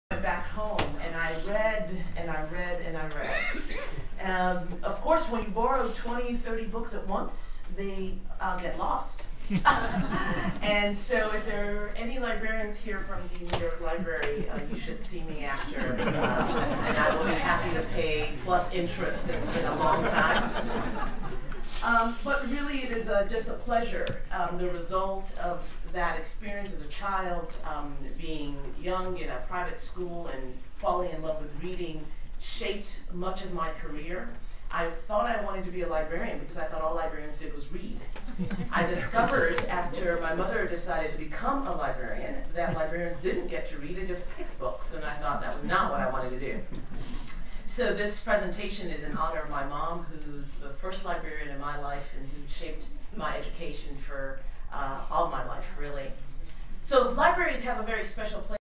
IFLApm2011_copyright-session
Ruth Okediji, William L. Prosser Professor of Law,
University of Minnesota Law School on "Copyright in context and environmental positioning" at the IFLA Presidential Meeting 2011 in the Hague.